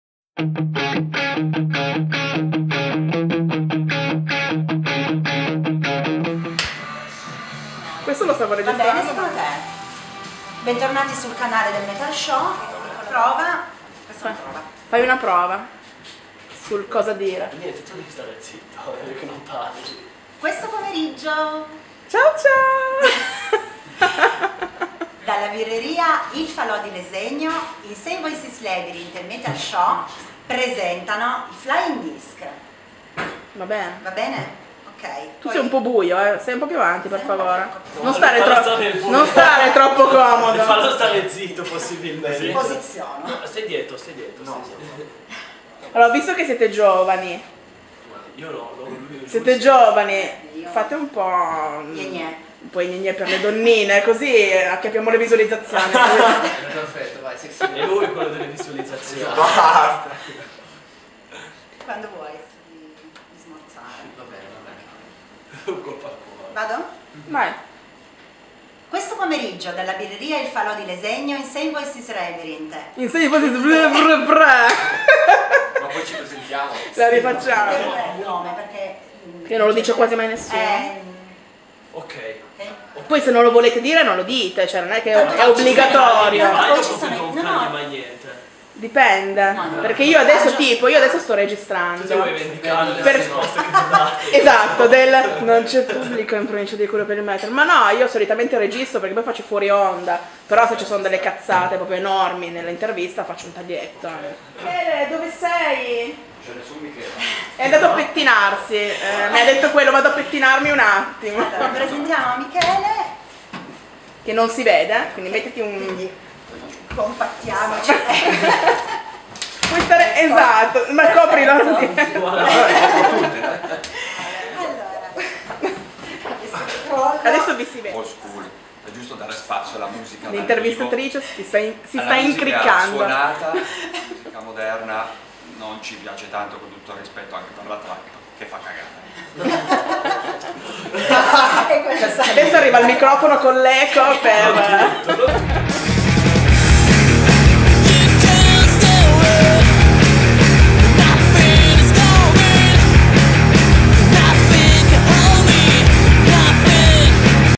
Flying Disk - Fuori onda
Alcuni fuori onda con i Flying Disk alla birreria Falò di Lesegno (CN)